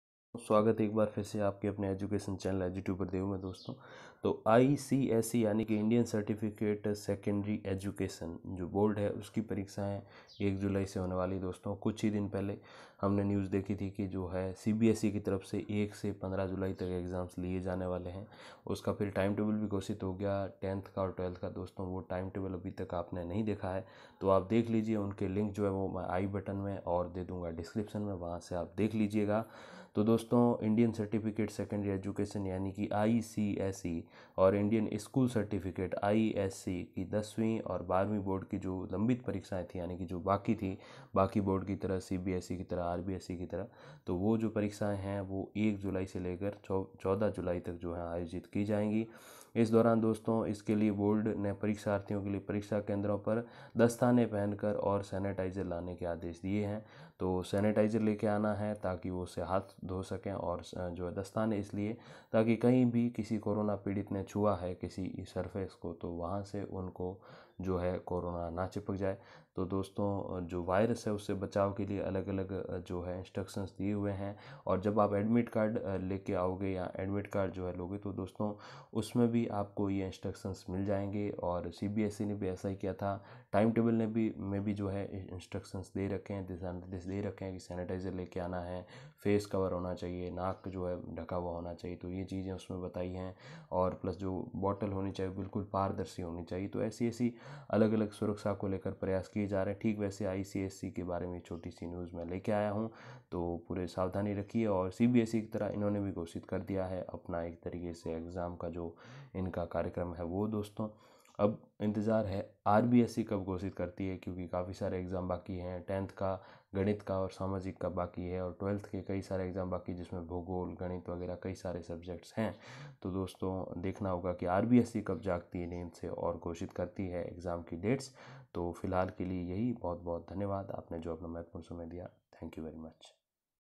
ICSE Board Exams from 1 July
Indian Certificate Secondary School- ICSE ki taraf se bhi CBSE ki tarj par baki pade board exams ko 1 july se aayojit karwaya jayega.